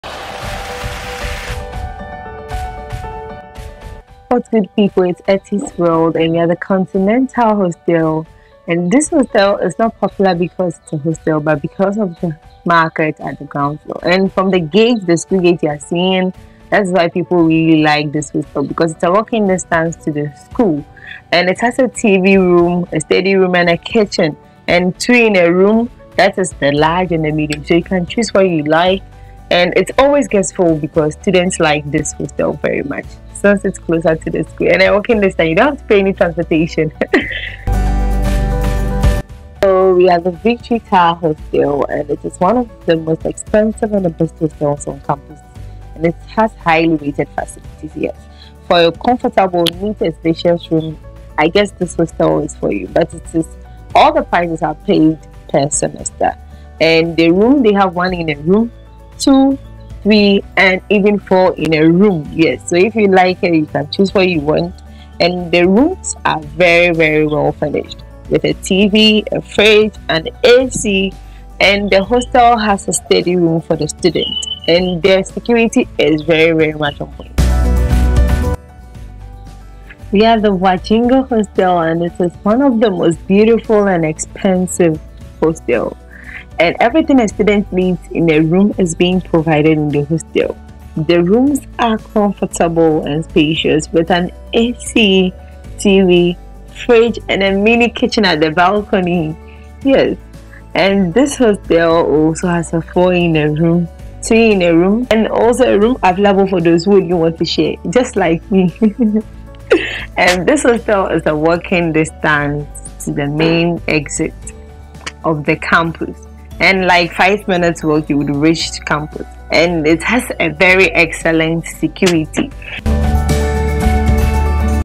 Expensive hostels? ||Wagyingo,Victory towers and popular continental Hostel super market at KNUST
KNUST Hall & Hostel Tour

- St Theresa hostel at KNUST

- Brunei Hostel for international student

- Queen Elizabeth || Hall at KNUST

- Unity (Conti Hall) at KNUST

- My first Visit at the Main Administration of KNUST